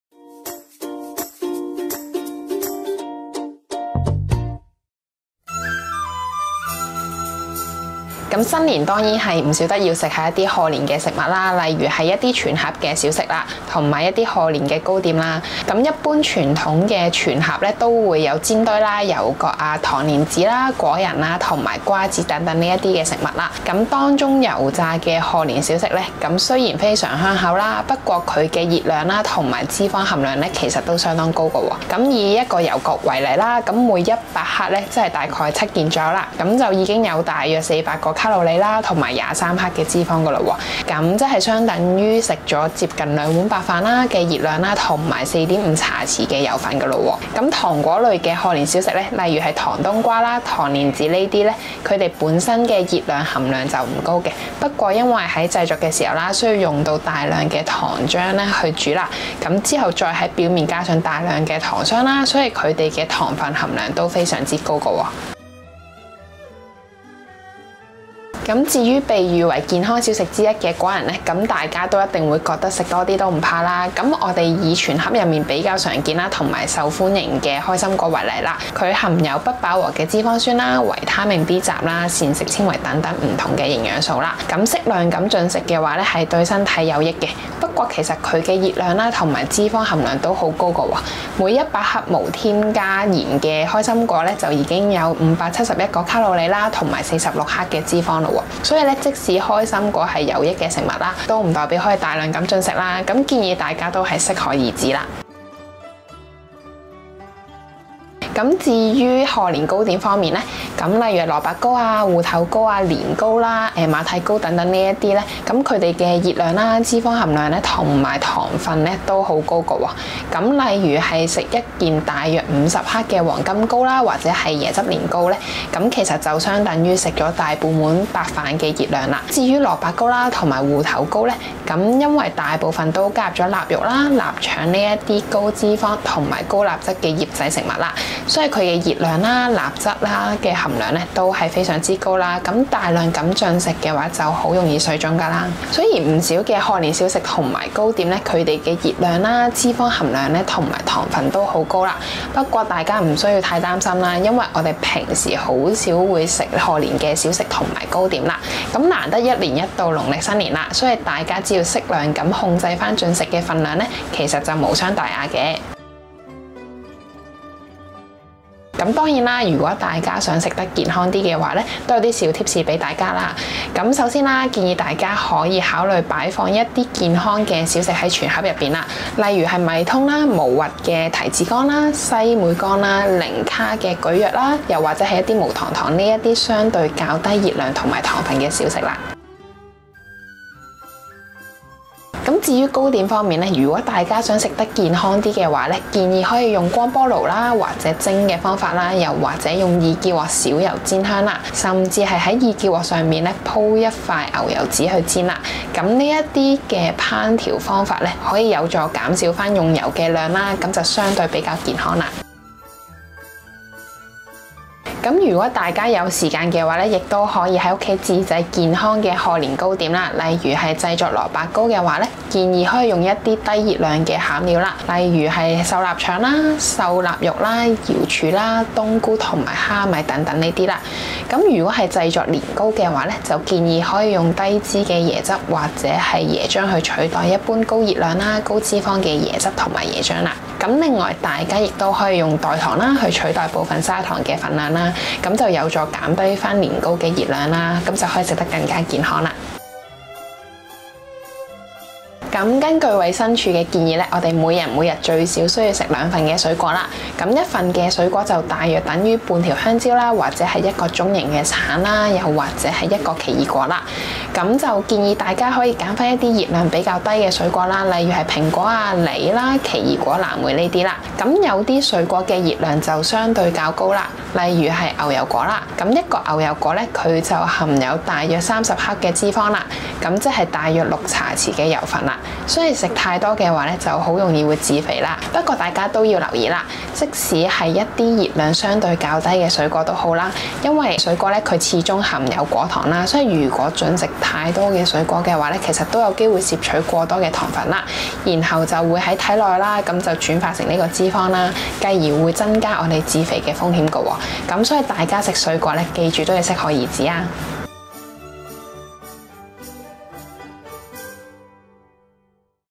送虎迎兔齊賀歲（上）：健康應節食品逐樣捉
虎年仲有幾日就成為過去，又到迎接玉兔的好日子。每年農曆新年都係脂肪爆炸的美食假日，各式各樣賀年食品例必吸引到大家，大開金口去擦餐飽。不過，原來賀年食品中，都有健康有營嘅選擇。在大時大節前，營養師温巧晴會為大家推介幾款健康嘅賀年食品，讓大家新嘅一年，個個笑口常開，豐衣足食。

延伸閱讀：
1）盆菜豐衣足食